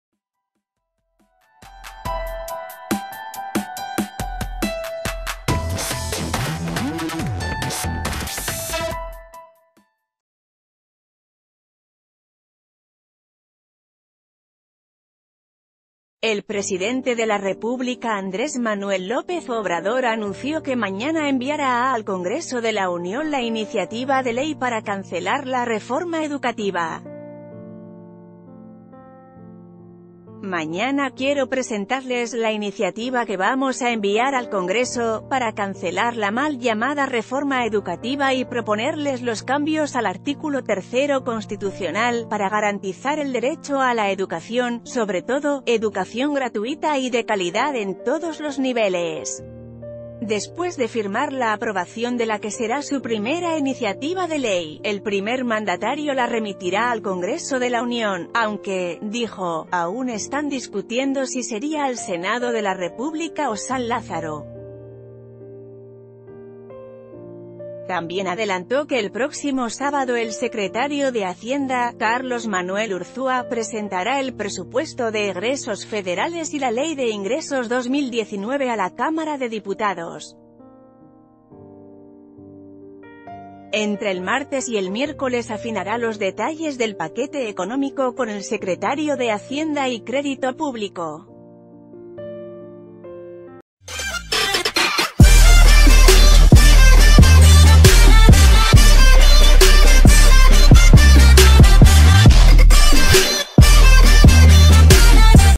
AMLO enviará mañana iniciativa para derogar Reforma Educativa
El presidente de la República Andrés Manuel López Obrador anunció que mañana enviará al Congreso de la Unión la iniciativa de ley para cancelarla Reforma Educativa. Mañana quiero presentarles la iniciativa que vamos a enviar al Congreso, para cancelar la mal llamada reforma Educativa y proponerles l...